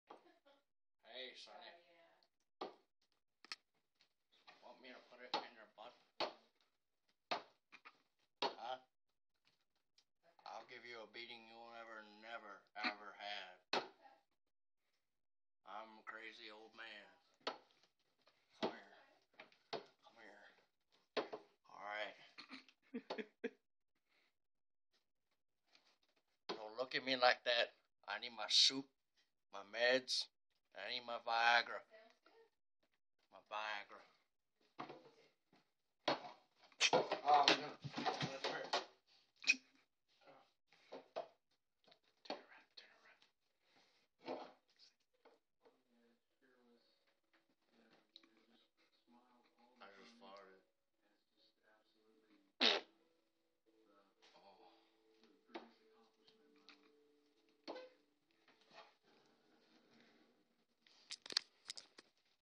The creepy crazy old man
Crazy old man needs his meds and doesn't like camera in his face